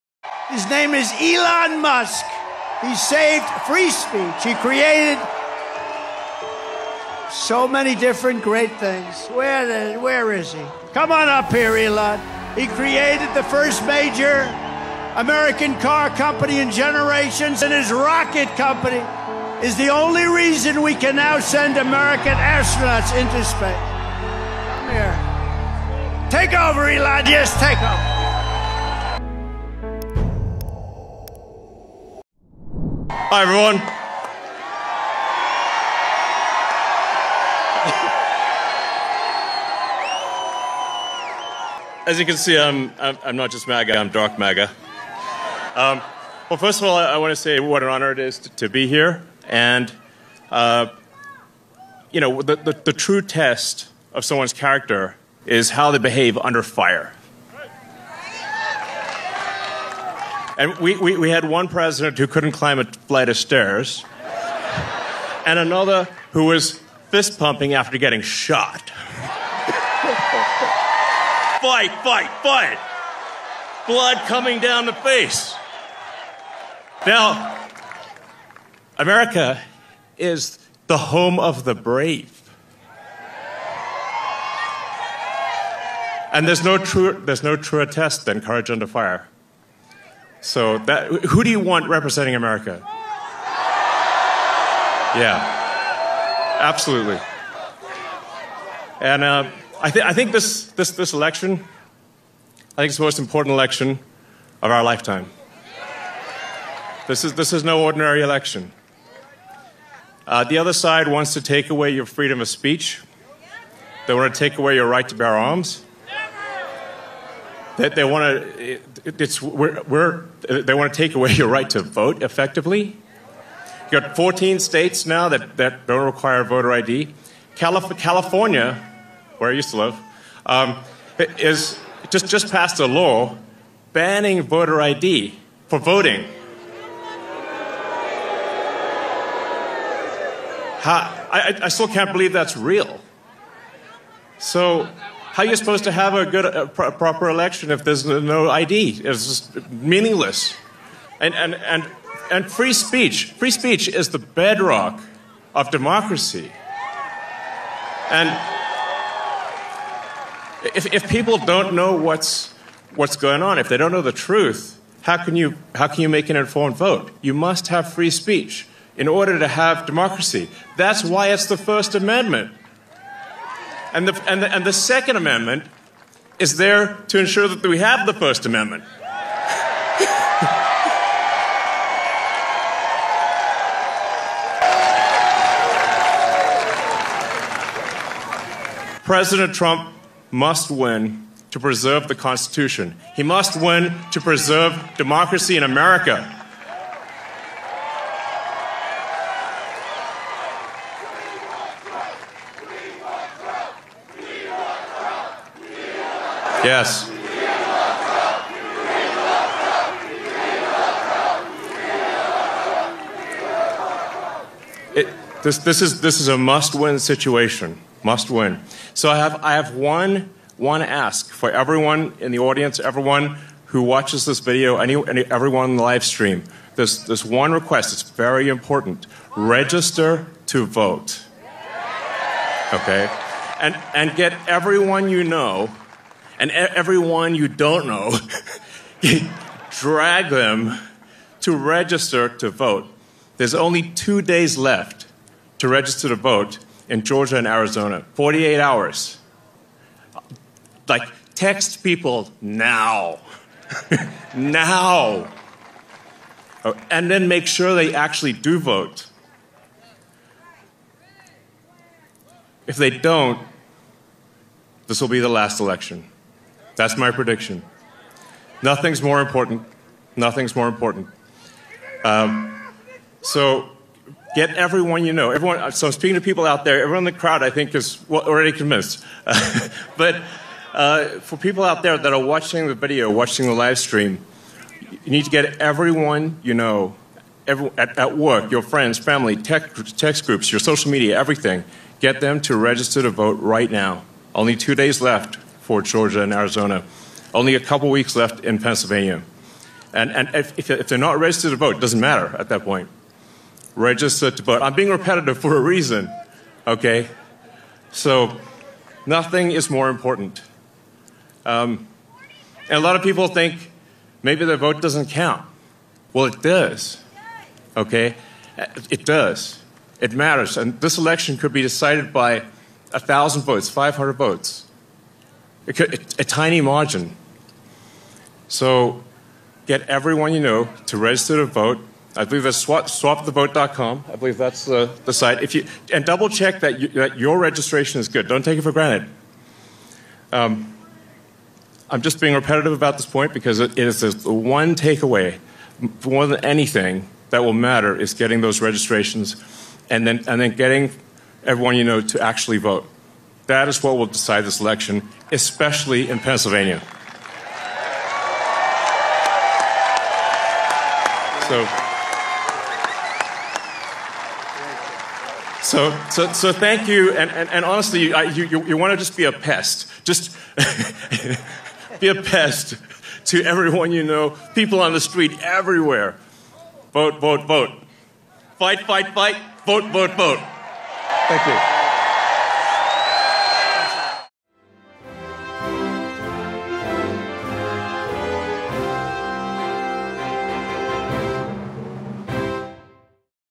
[英語ニュース] 革新のアイコン大統領候補応援演説|イーロン・マスク | Elon Musk |日本語字幕 | 英語字幕
イーロン・マスク氏が第45代アメリカ合衆国大統領であるドナルド・トランプ氏を力強く応援し、支持を訴える応援演説です。マスク氏は、トランプ氏の政策や実績について言及し、彼を次期大統領候補者として強く支持しています。さらに、マスク氏は民主主義を守るための重要な要素として、言論の自由に触れています。彼は、自由で公正な報道が民主社会の基盤であり、特にこの選挙でその価値が強く問われていると強調しました。

国民一人一人が投票登録する重要性を訴え、必ず投票に参加するよう呼びかけました。マスク氏がトランプ氏を支持する理由とは何なのか、彼の話を聞いてみましょう。今日も楽しんでください。

2)  This video modifies an original work by adding texts to the work.

3) This video borrows very small bits of material from an original work.

🔵ATTRIBUTION 🔵